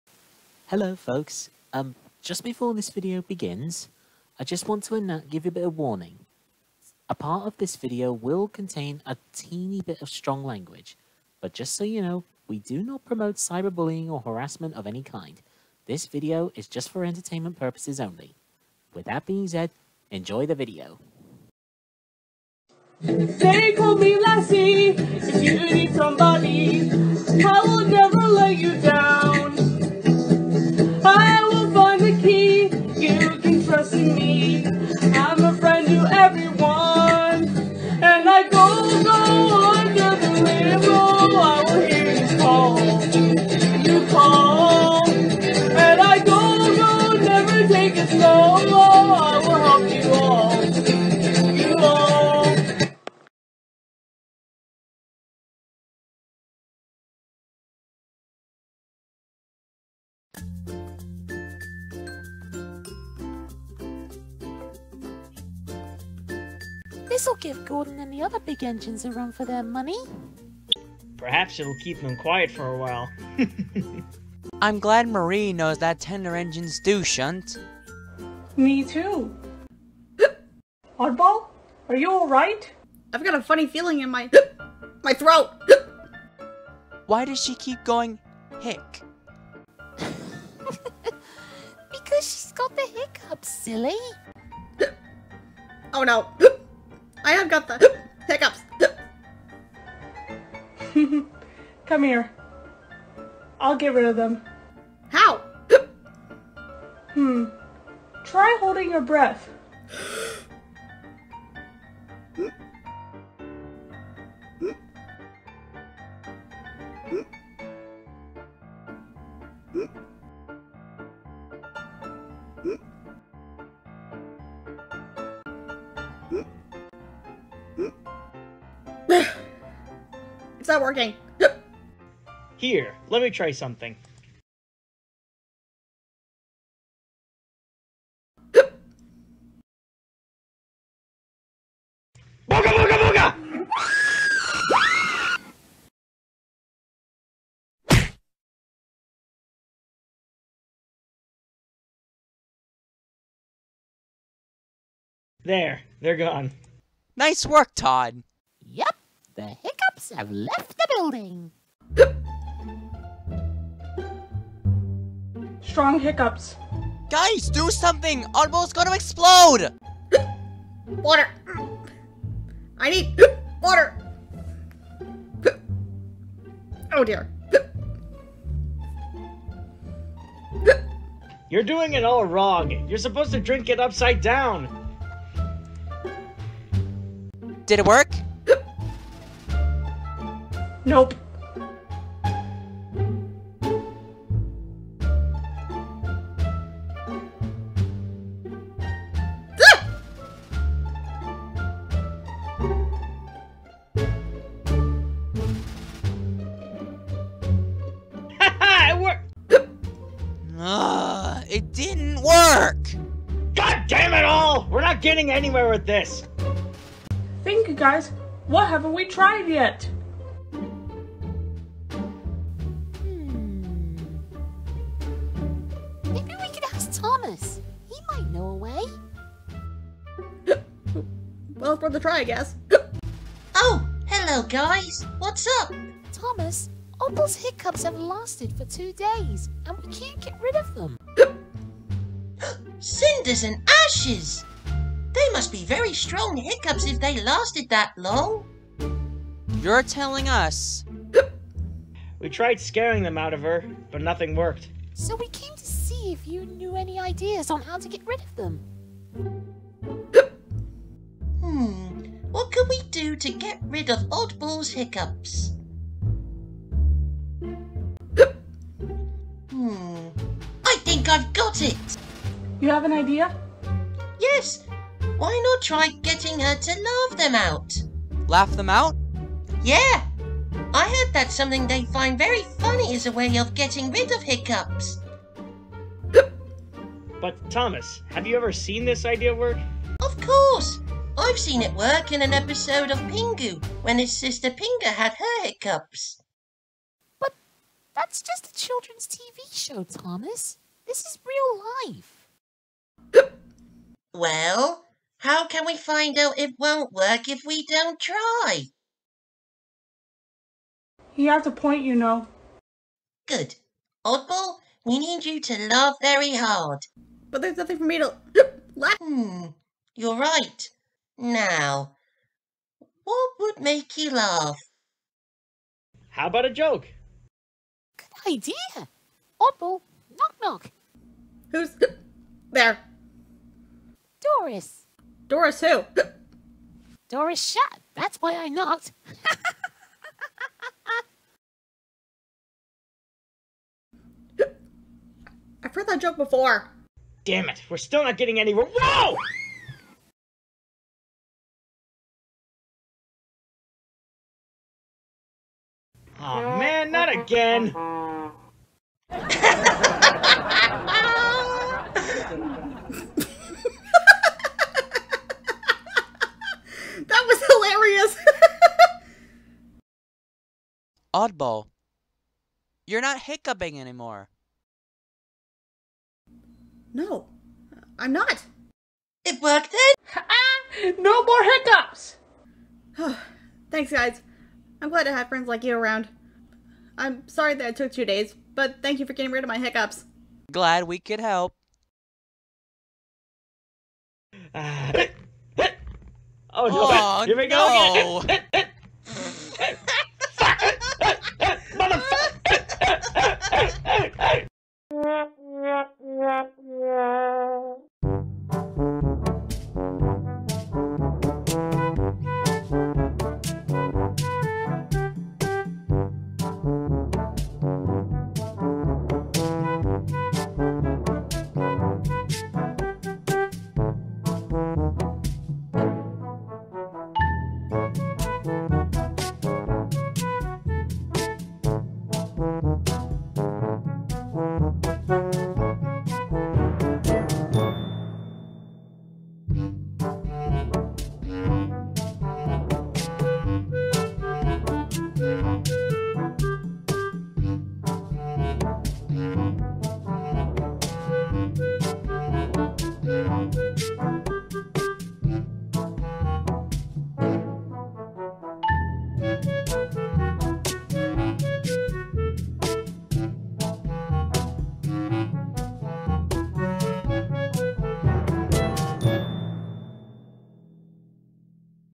TOBR: The Tales Of Bambi And Lassie EP 5: Hiccups
Warning: There will be a tad bit of strong language at the end of this episode.

In this fifth episode, poor Oddball has ended up with hiccups, so now it's up to Bambi, Lassie, Todd and Vixey to help her get rid of them, but most of their attempts don't go so well.

Voices
Daniel Wells
TheNPRRE2
OliviaDragonGirl
Geo The Avatared Creature
Marclovestrainz
Wild Cat Studios

Intro made by Olivia Bickley (OliviaDragonGirl).

Bambi (Bambi Franchise), Todd and Vixey (The Fox And The Hound), And Oddball (101 Dalmatians Franchise) Belongs to Disney.

Lassie by Eric Knight/DQ Entertainment.

Locomotive Models By Hornby And Bachmann.